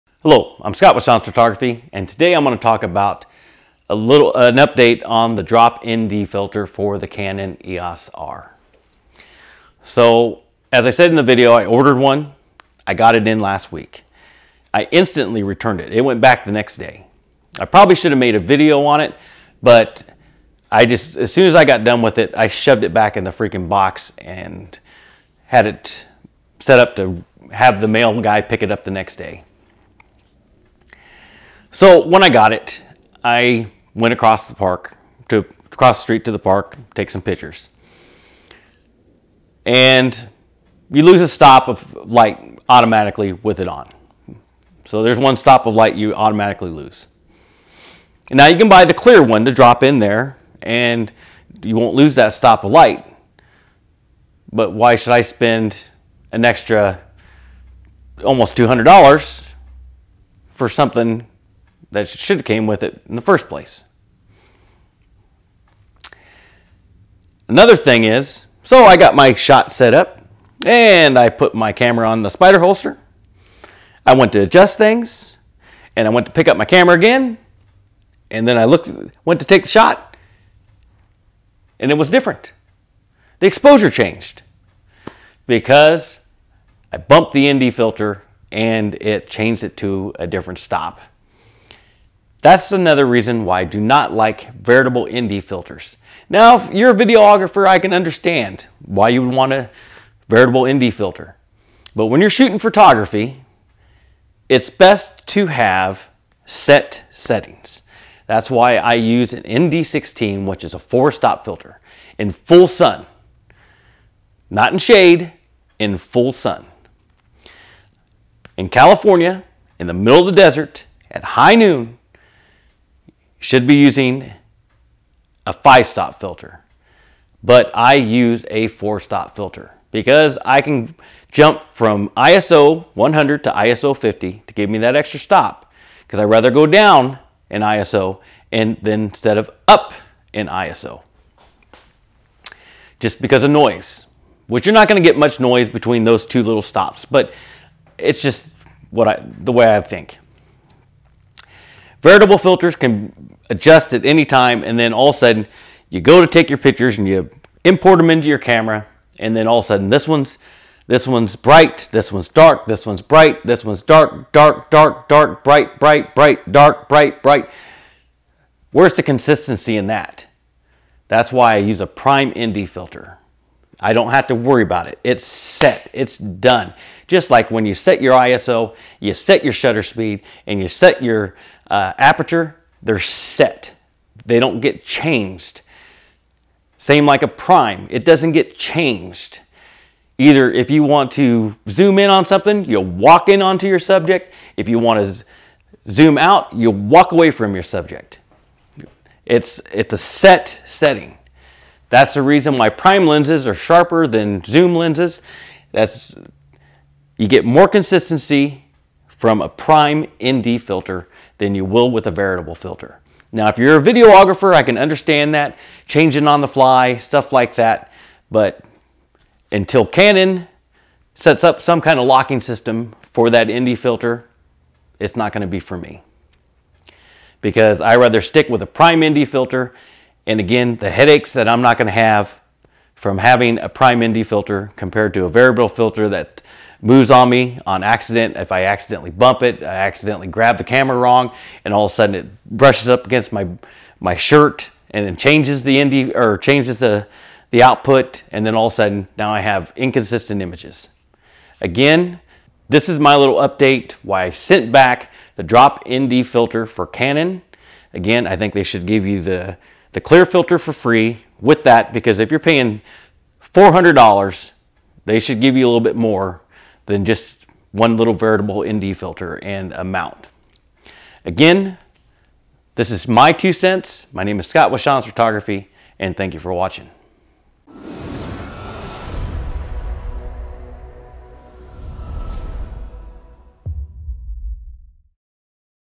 Saying No To The Canon Drop In Filter Mount Adapter for Canon EOS R
Saying No To The Canon Drop In Filter Mount Adapter for Canon EOS R; Till They Fix Or Start Selling Prime Filters